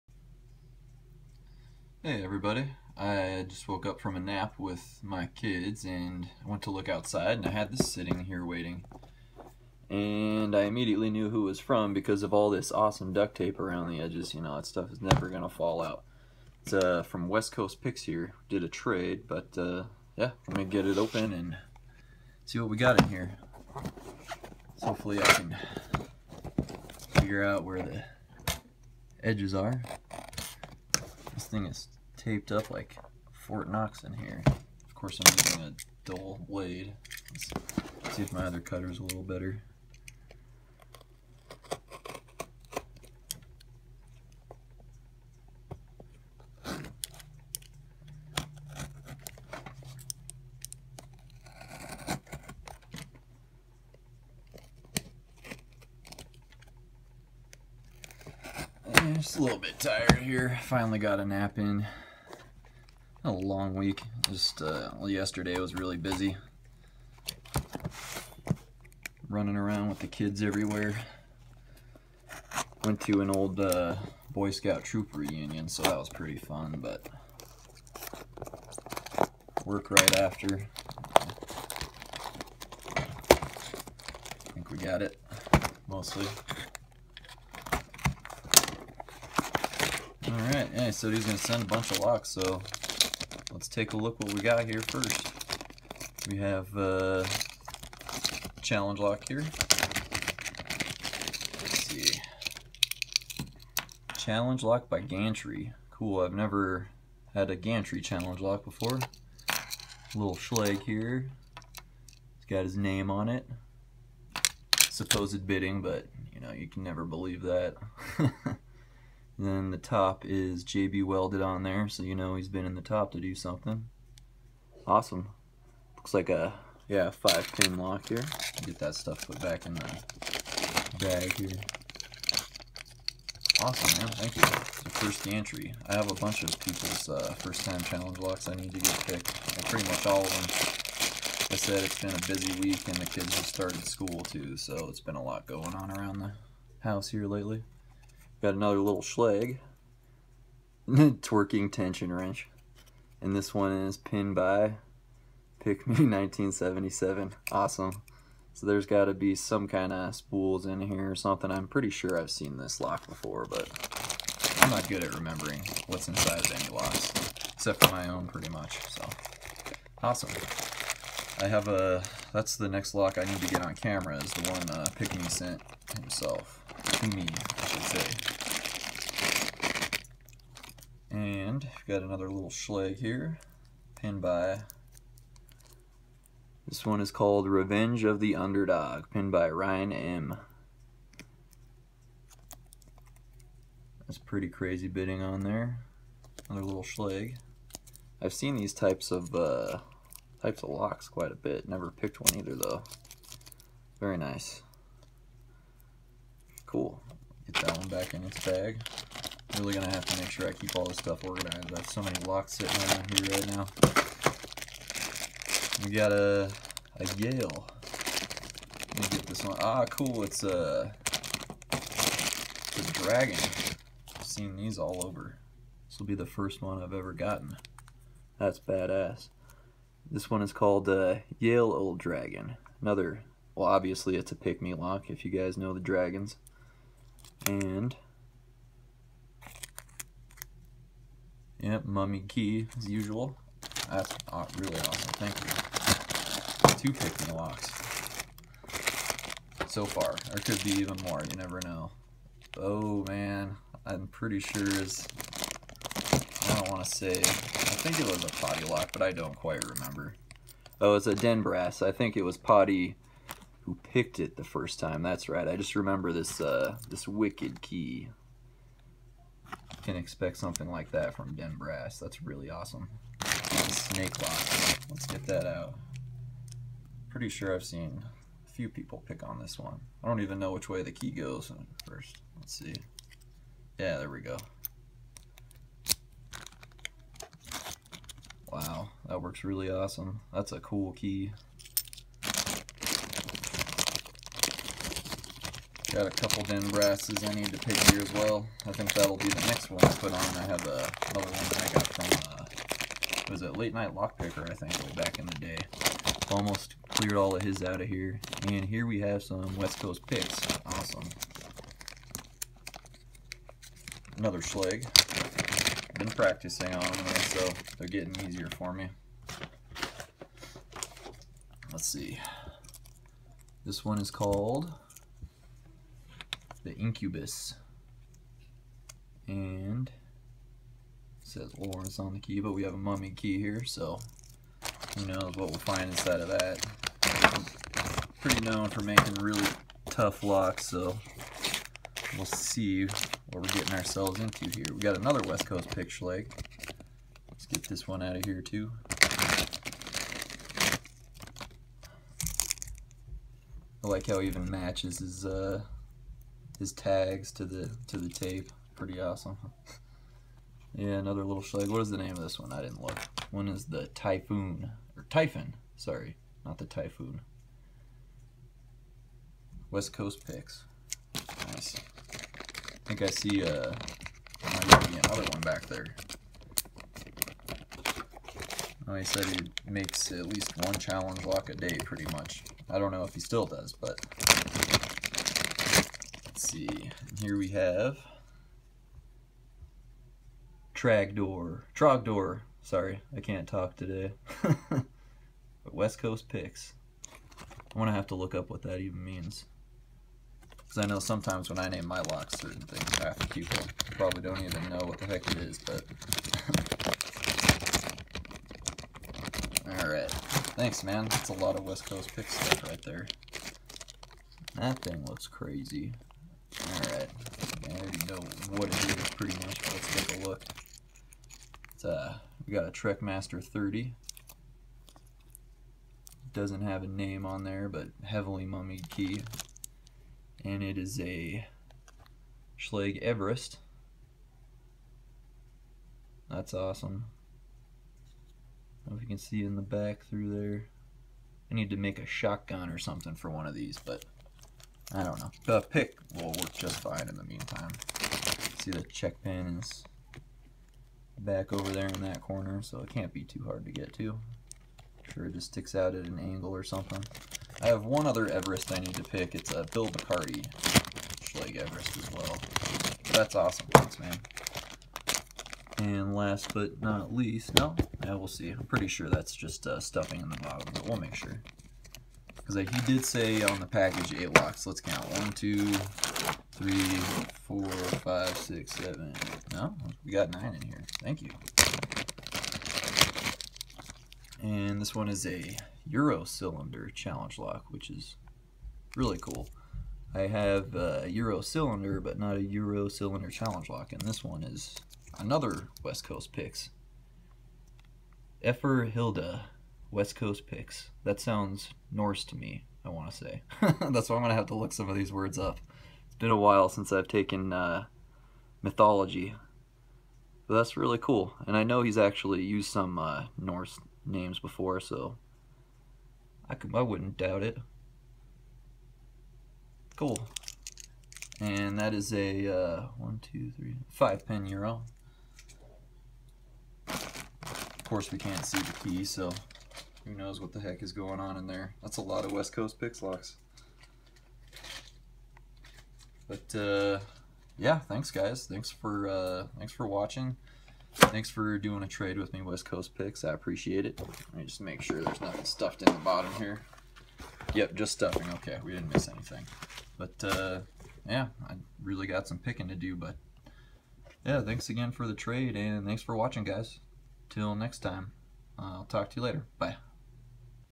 Awesome package from West Coast Picks! (19)
Hey guys, welcome back.  This is the opening up of a great package of challenge locks from West Coast Picks!